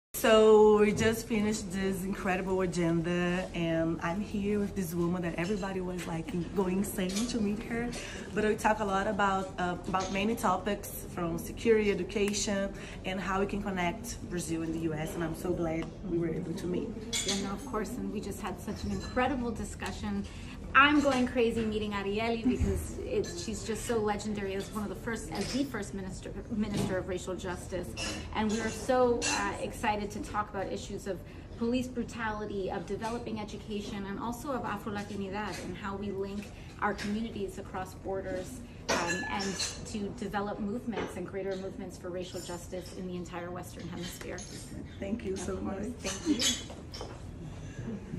ANIELLE FRANCO E OCASIO-CORTEZ SE ENCONTRAM PARA DISCUTIR IGUALDADE RACIAL NO OCIDENTE
A ministra da Igualdade Racial, Anielle Franco, se encontrou com a congressista democrata dos Estados Unidos, Alexandria Ocasio-Cortez. Em vídeo, afirmaram que discutiram tópicos de segurança, educação e como tornar o desenvolver movimentos pela igualdade racial no ocidente.